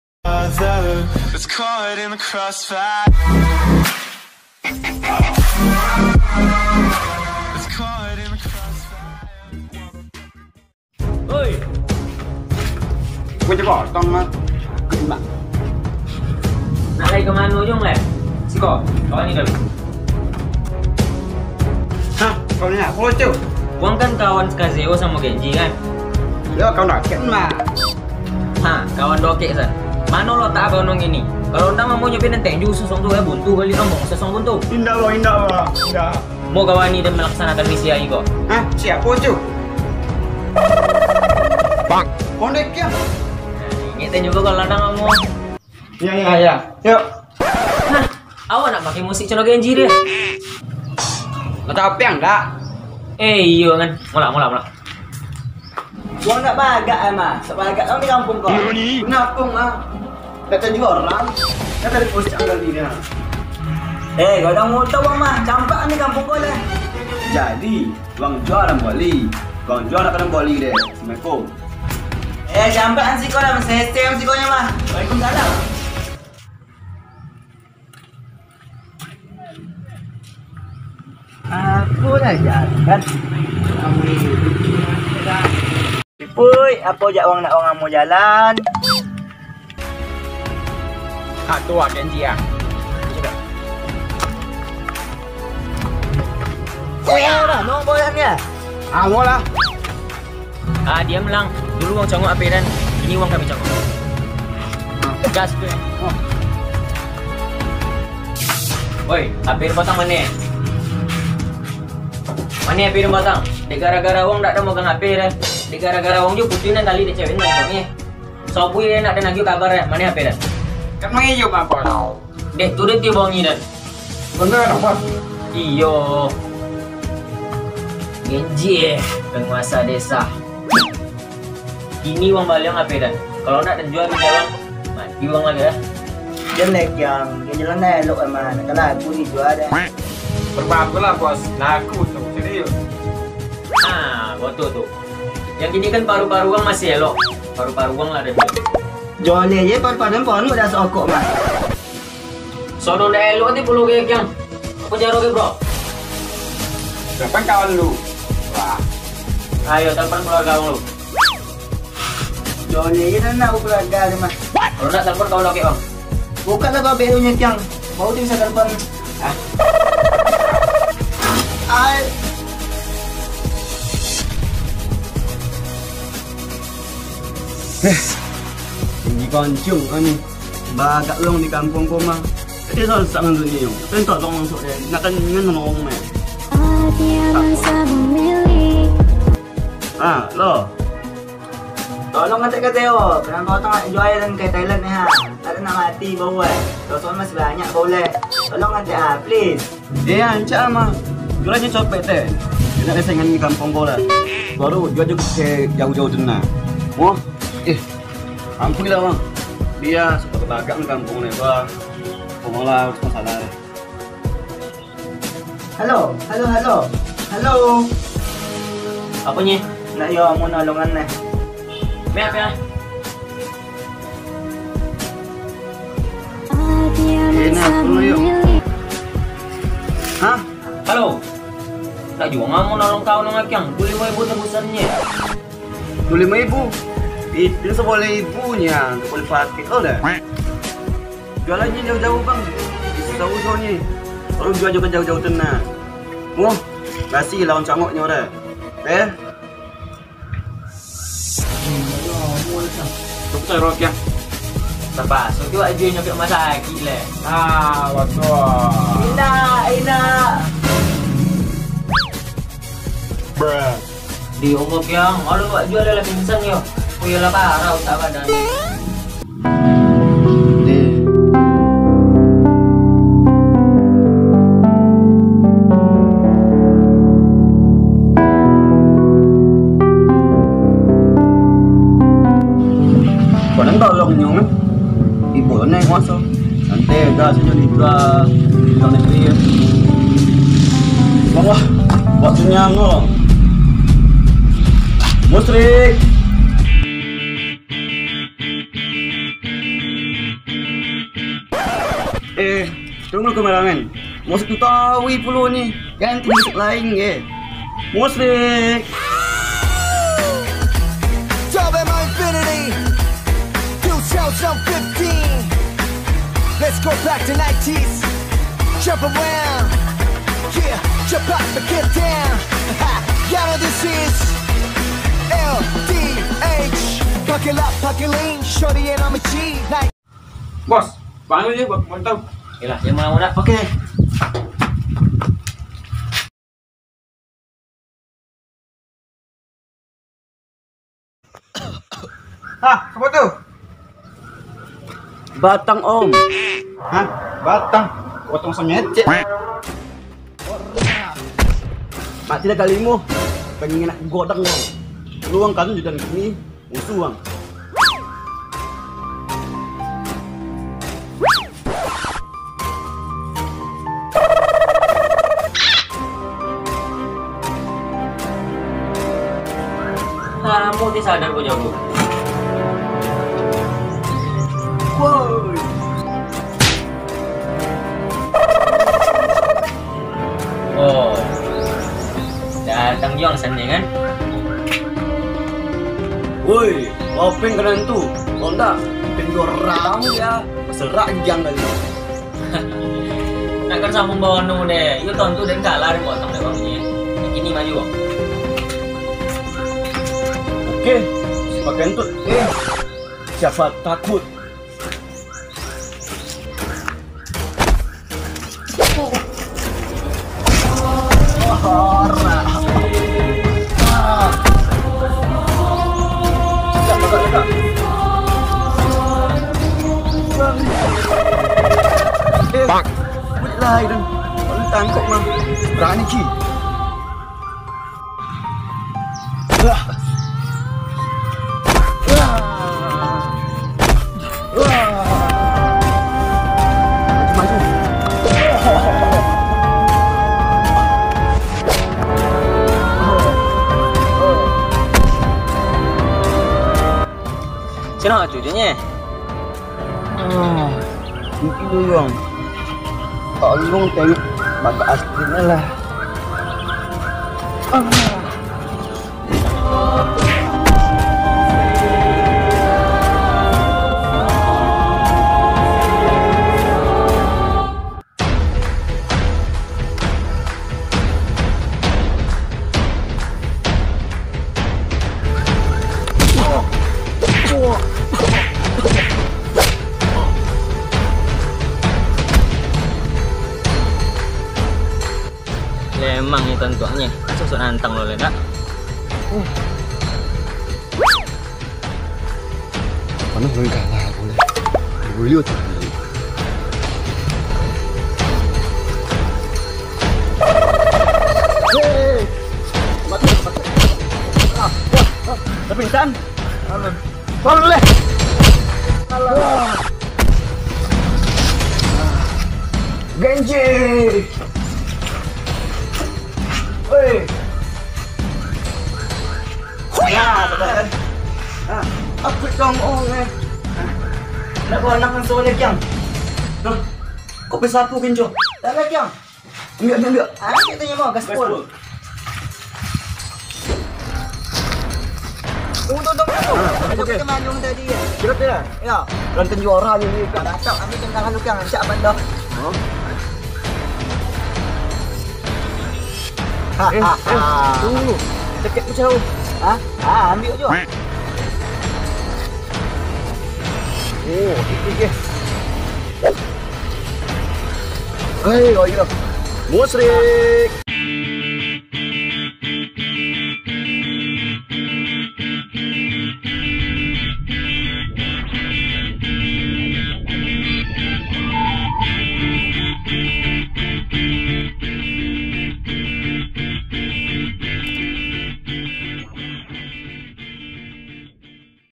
Misi penyelamatan Genjie || lawak ocu
assalamualaikum guys,ini bercerita seorang preman yang mencuri genji dan ingin menjualnya,akhirnya kuburan kazeo datang untuk menolong nya, seperti apakah ceritanya saksikan video berikut ini

*misipenyelamatangenjie